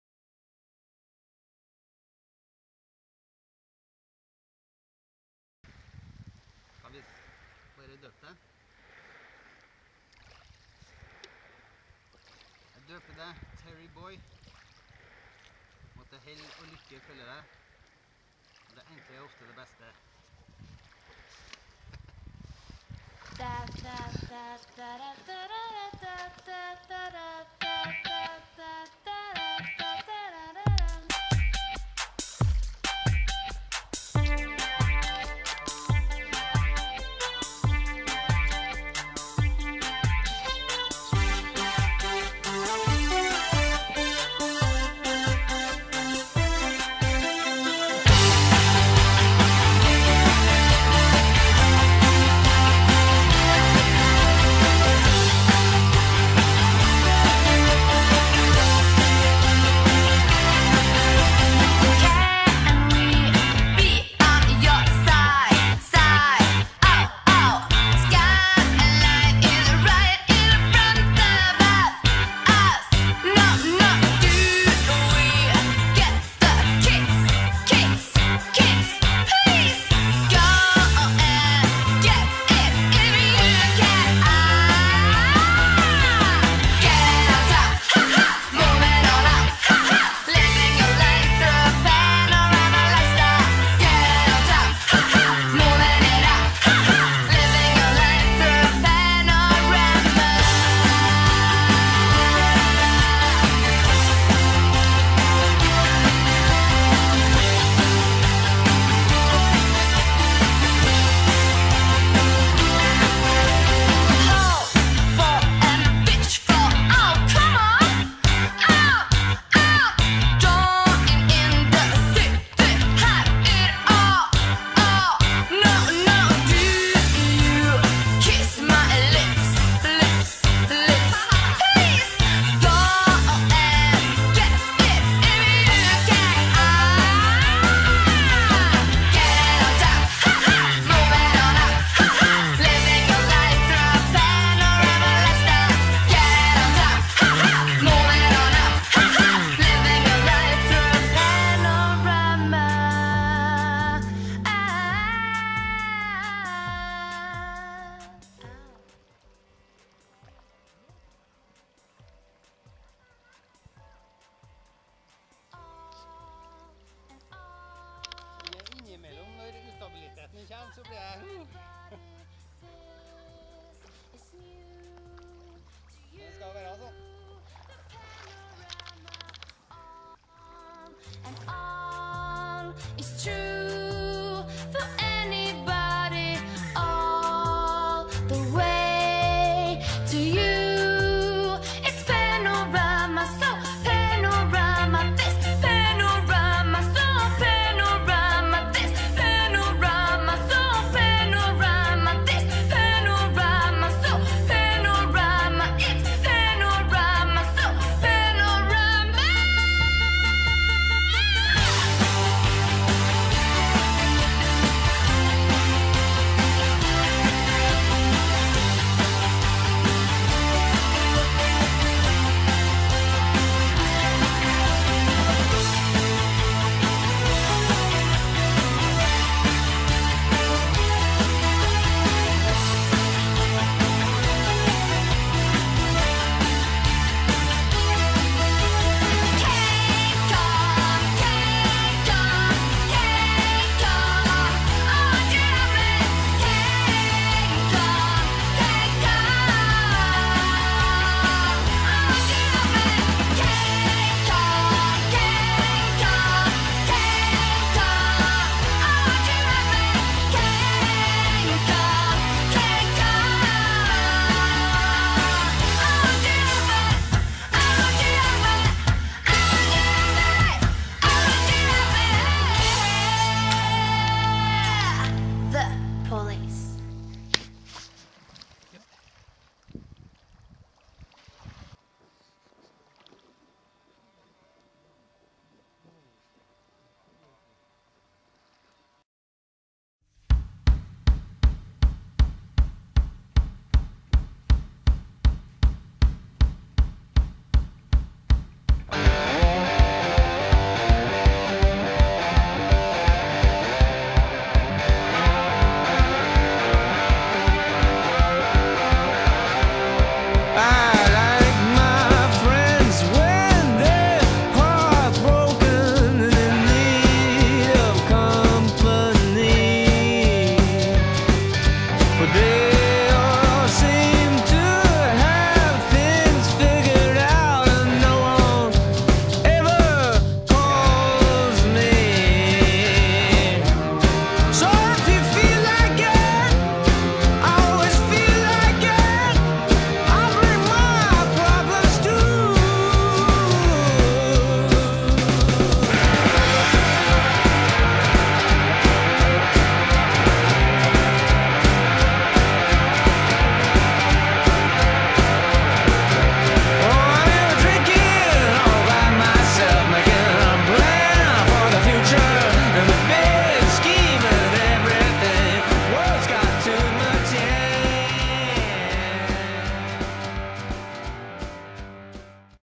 Greenland kayak
Greenland Kayak - Launching of the TerryBoy, custommade/ selfmade westGreenland kayak at Korsvika Trondheim, feb 2016